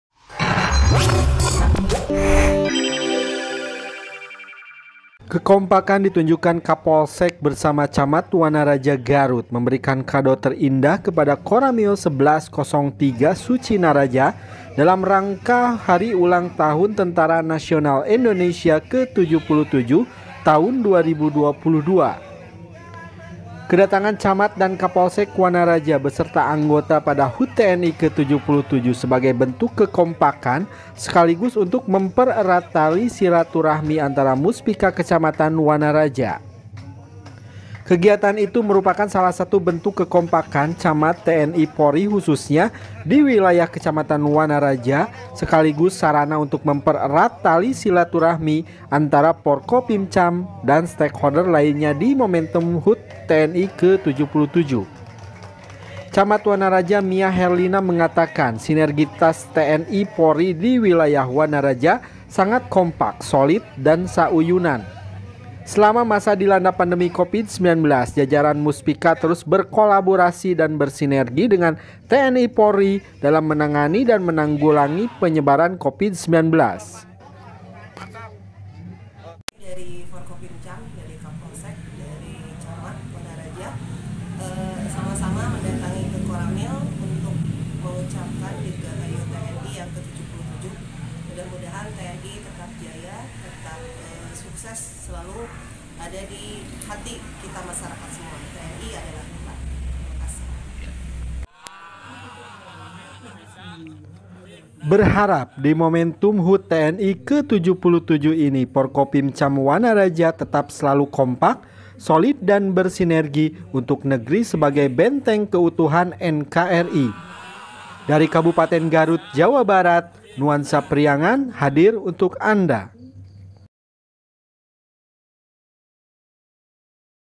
Kompak...!!! Kado Terindah Hut TNI Ke 77 Forkopimcam Wanaraja Garut
KAB.GARUT. NUANSAPRIANGAN-TV - Kekompakan ditunjukkan Kapolsek bersama Camat Wanaraja Garut, memberikan Kado terindah kepada Koramil 1103/Sucinaraja dalam rangka Hut TNI Ke 77 Tahun 2022. Rabu (5/10/2022).

Kedatangan Camat dan Kapolsek Wanaraja beserta anggota pada HUT TNI Ke-77 sebagai bentuk kekompakan sekaligus untuk mempererat tali silaturahmi antara Muspika Kecamatan Wanaraja.

Kegiatan ini merupakan salah satu bentuk kekompakan, Camat, TNI - Polri khususnya di wilayah kecamatan Wanaraja, sekaligus sarana untuk mempererat tali silaturahmi antara Forkopimcam dan Stakeholder lainnya di Momentum Hut TNI Ke 77.

Camat Wanaraja Mia Herlina mengatakan, sinergitas TNI/Polri di wilayah Wanaraja sangat kompak, solid dan sauyunan. Selama masa dilanda pandemi Covid-19 jajaran Muspika terus berkolaborasi dan bersinergi dengan TNI - Polri dalam menangani dan menanggulangi penyebaran Covid-19.

Berharap di momentum HUT TNI ke 77 itu Forkopimcam Wanaraja tetap selalu kompak, solid dan bersinergi untuk negeri sebagai benteng keutuhan NKRI.